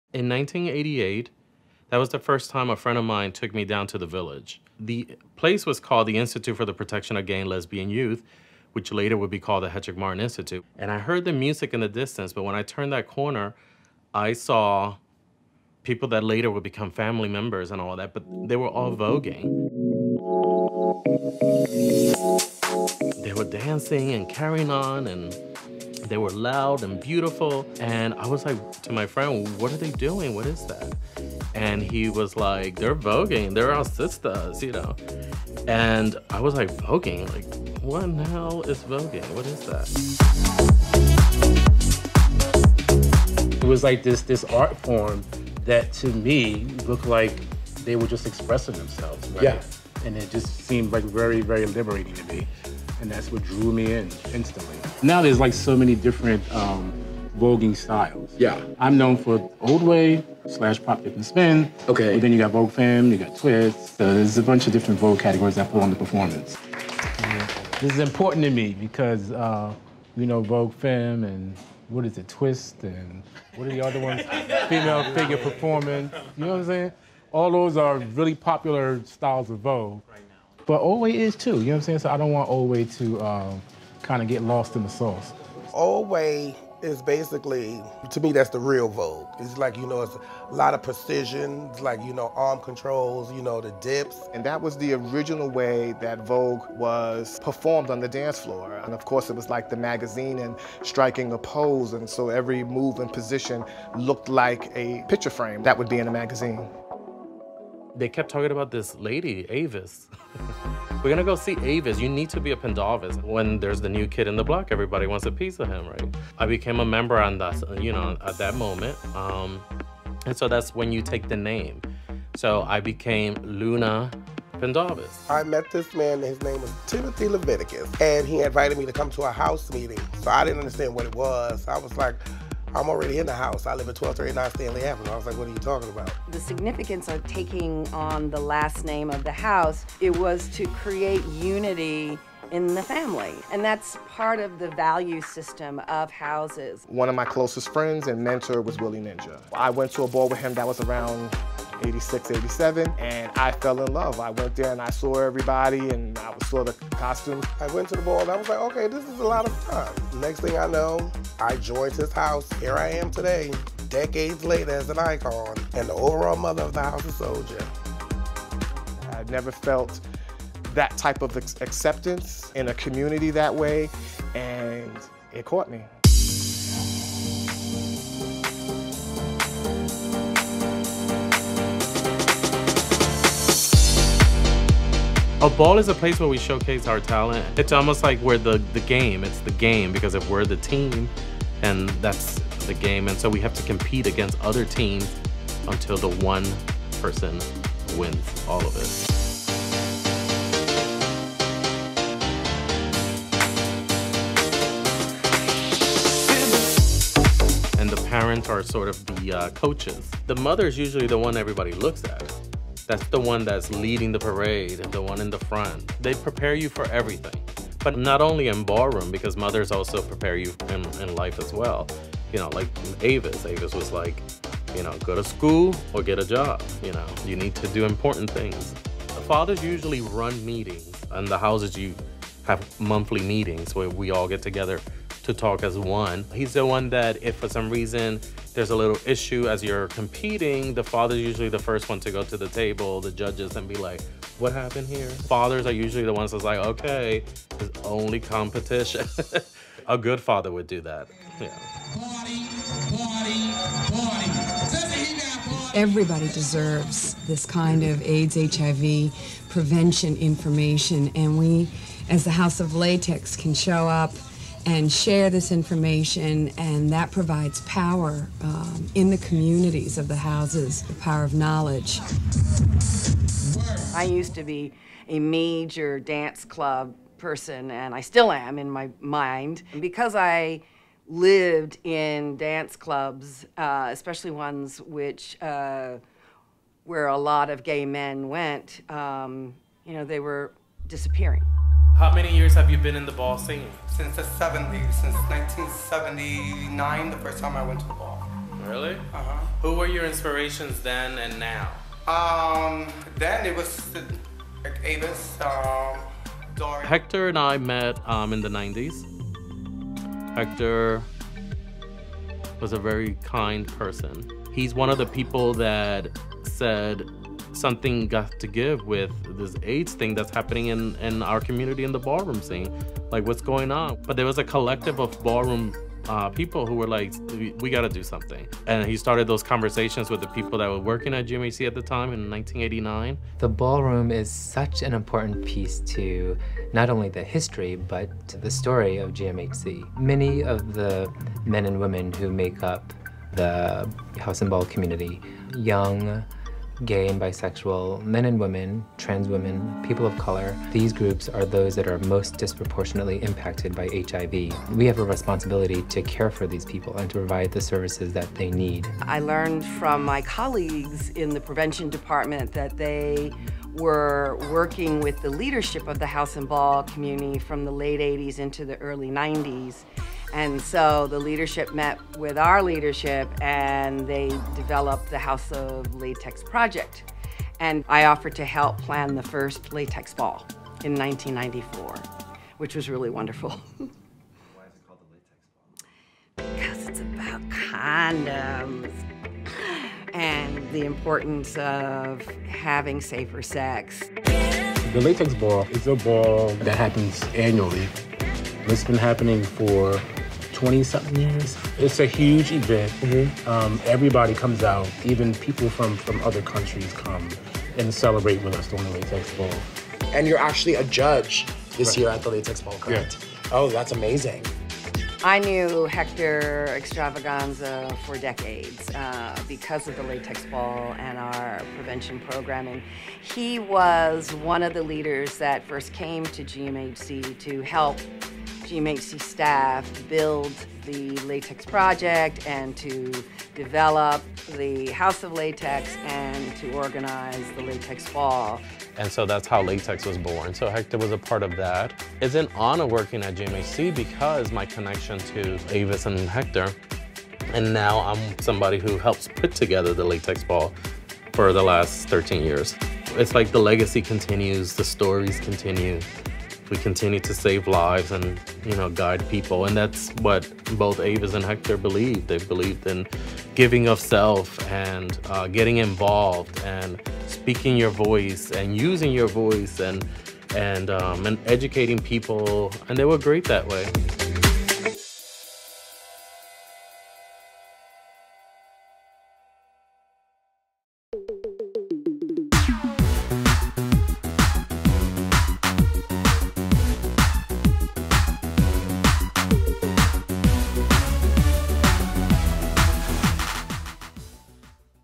From Paris to Pose | Latex Ball | Logo TV
Logo speaks with members of the ballroom community and GMHC on their history with voguing, creating TheLatexBall, how and why houses were formed, the HIV/AIDS crisis, and those who paved the way.

Stay up-to-date with fierce content before and after the ball on Logo’s social media and LogoTV.com!

The Latex Ball is the largest gathering in the world for the House and Ball community, and attendees include hundreds of the community’s members who participate in the festivities; fashion models, designers, renowned and photographers; and leadership from community-based organizations.

More than 2,000 people from around the world are expected to attend the 29th annual Latex Ball, produced by Gay Men’s Health Crisis (GMHC). Taking place on Saturday, June 22, at Terminal 5 in New York City.